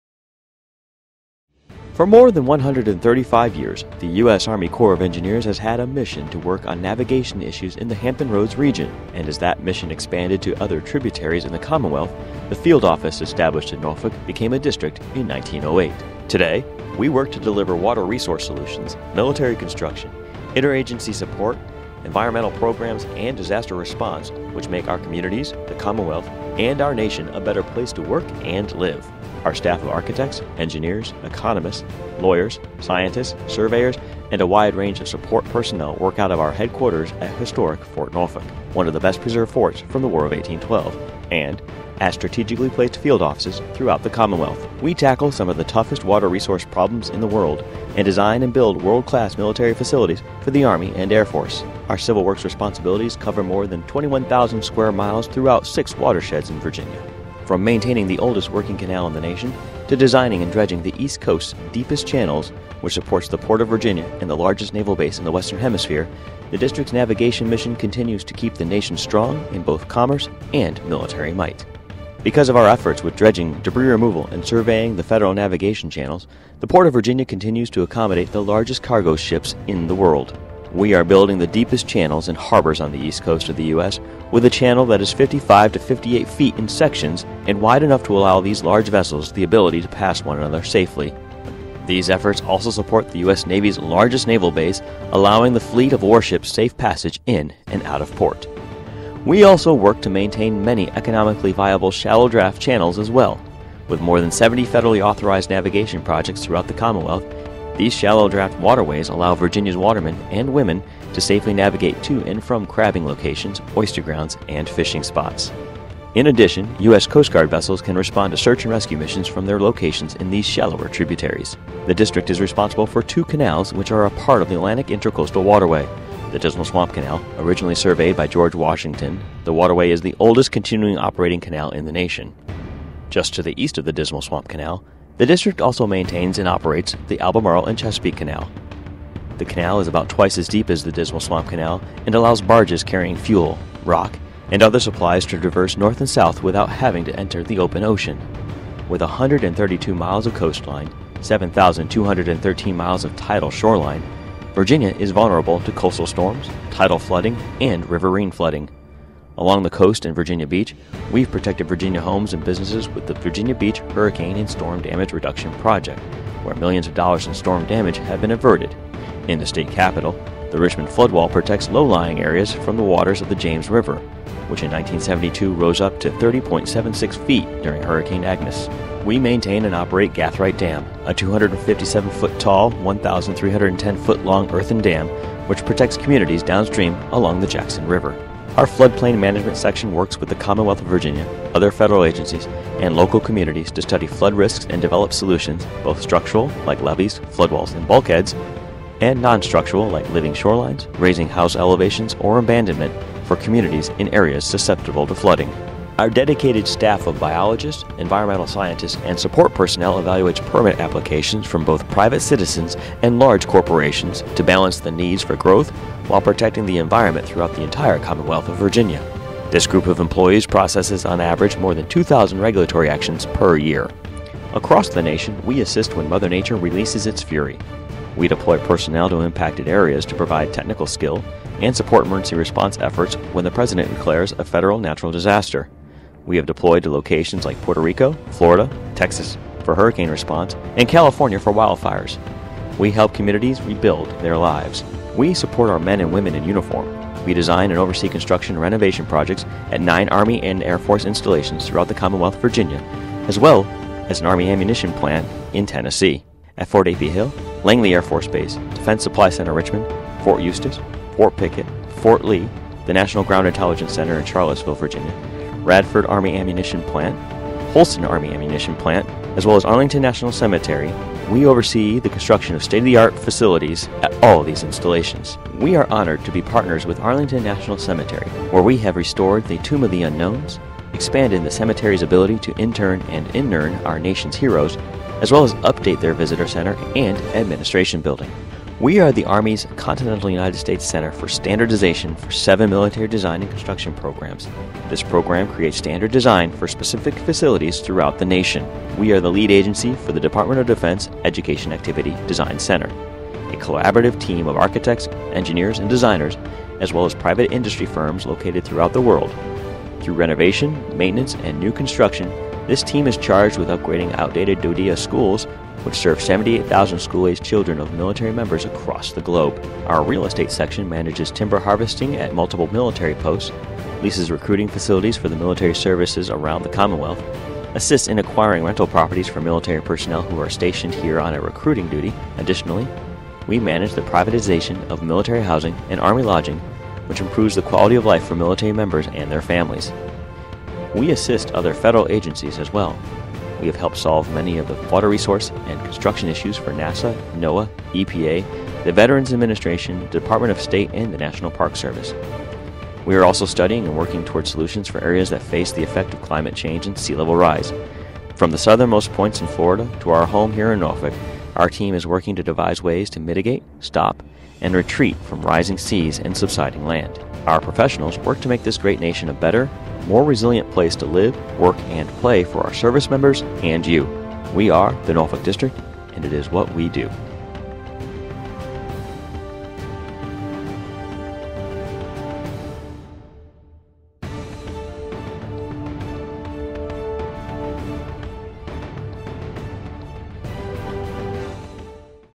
Norfolk District Mission Video
Since 1879, the Norfolk District, U.S. Army Corps of Engineers, has served the Nation, the Commonwealth of Virginia and its communities. Learn more about our diverse missions and the people who serve YOU.